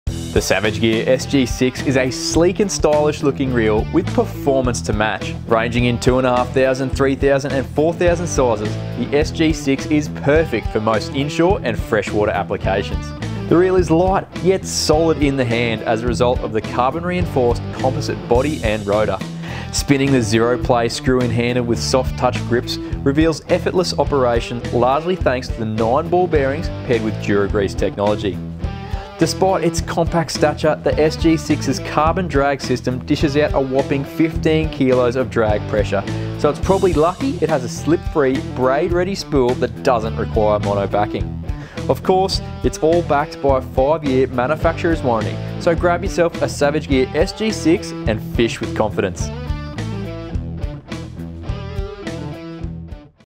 Introducing the Savage Gear SG6 Spinning Reel
Run your line out quickly and smoothly with the Savage Gear SG6 Spinning Reel. Made with quality materials it is strong, durable and the perfect match for your favourite Savage Gear rod. This high performance reel runs out smoothly to get your line and lure further. It has a strong carbon body and rotor that work to smoothly spin your line out on a 8+1 steel ball bearing setup which includes 2 high quality Japanese ball bearings. The 6:2:1 gear ratio lets you reel in lures quickly to trigger a reaction strike or make it easy to move to another spot. Easily control how much line fish get away with the sealed carbon drag system and reel in your line on the non-slip aluminium spool.